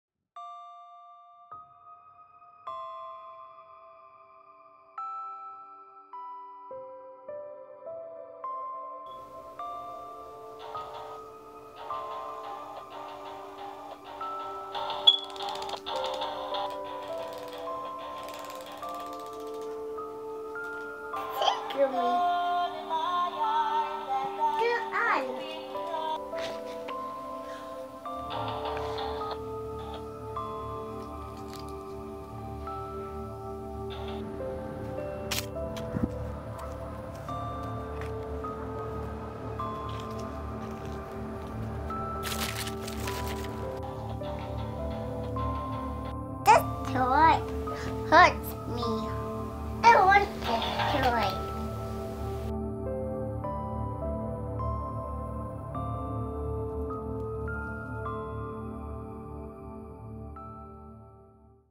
Meadow Scary Sequence
a short scary sequence that i shot of my daughter that is supposed to work as just some test footage for a horror short i am trying to write and shoot for a competition.